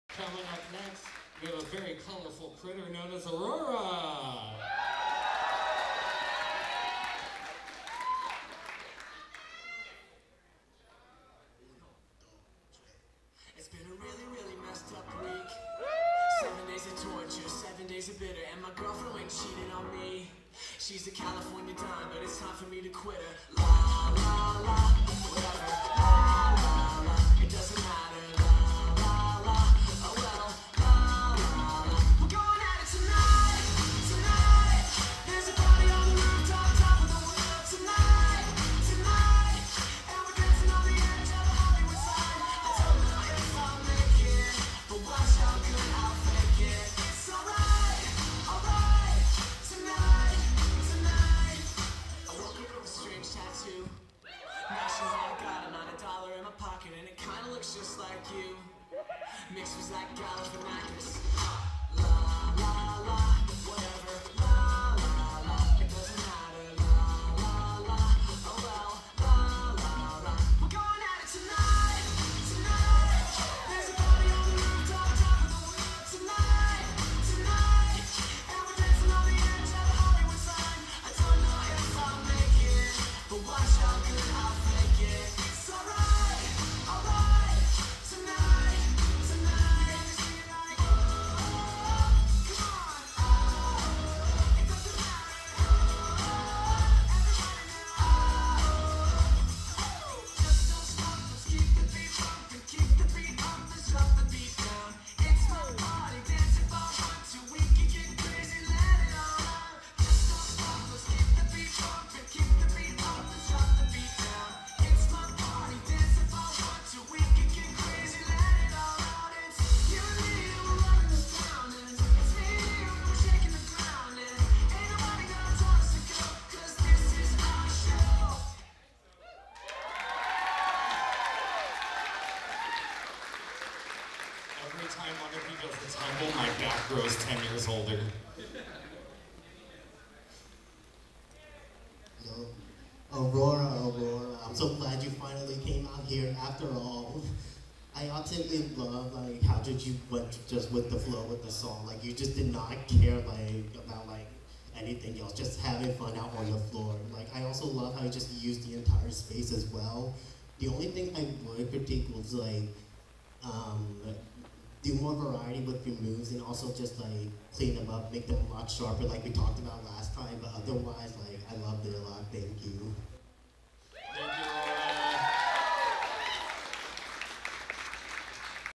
Aurora - Fursonacon 2022 Dance Competition
Fursonacon dance competition presented by Haus of Neon

Special thanks to all of our wonderful attendees. Y'all are what makes Fursonacon so special.